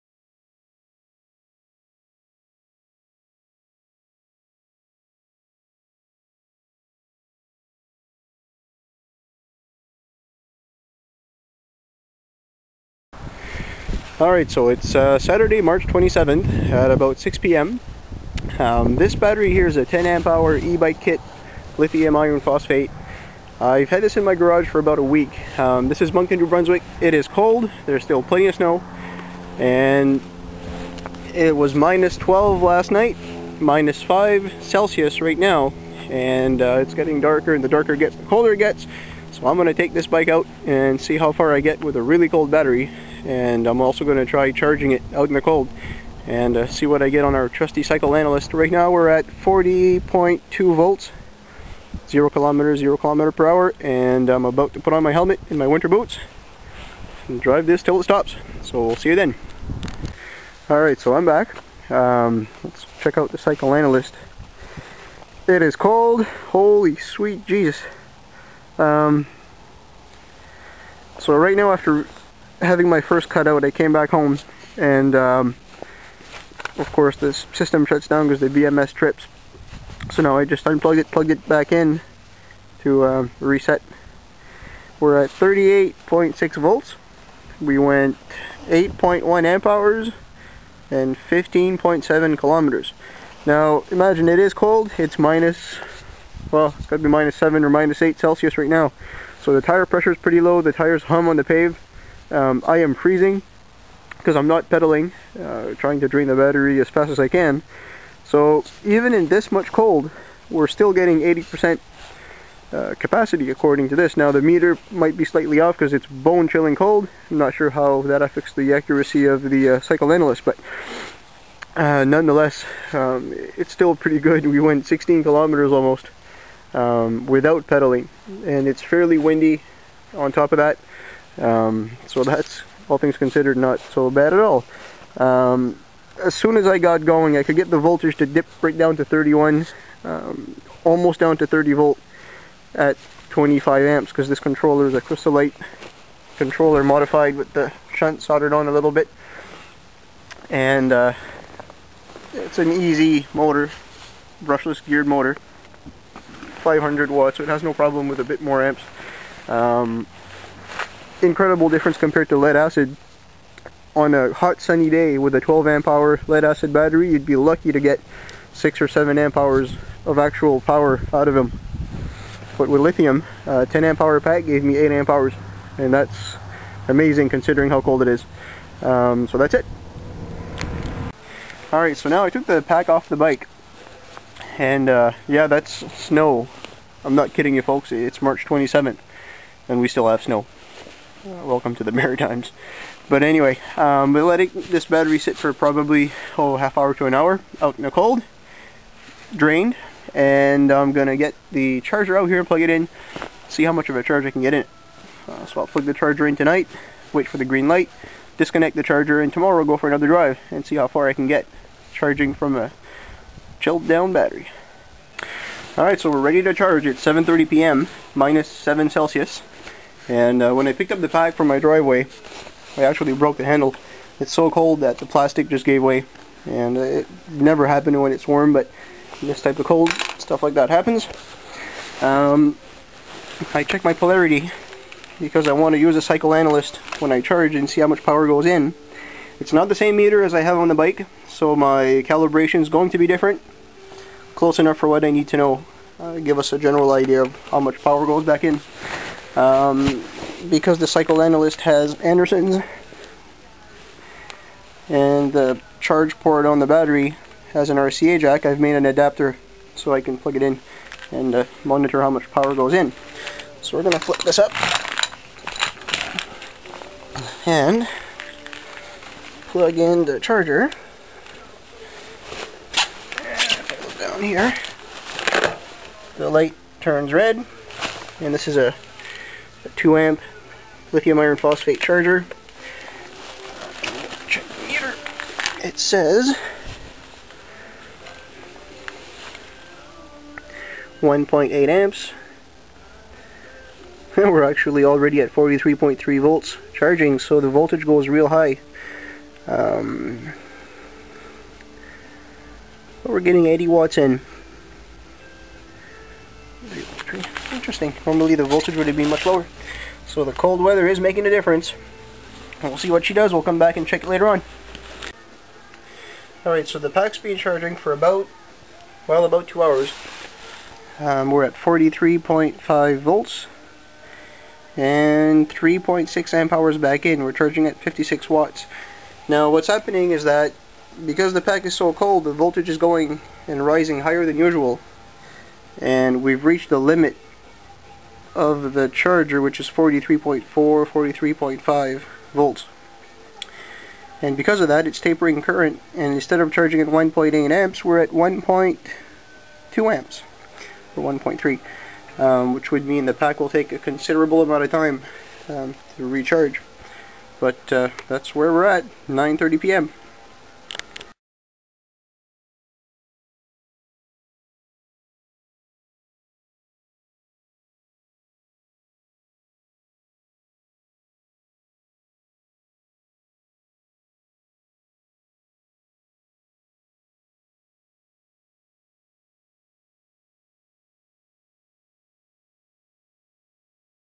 E-Bikekit.com LIthium battery cold weather testing
This is a 36v 10ah LiFePo4 battery pack i have been using fo a year now in all sorts of conditions, in sub zero temperatures this pack still works !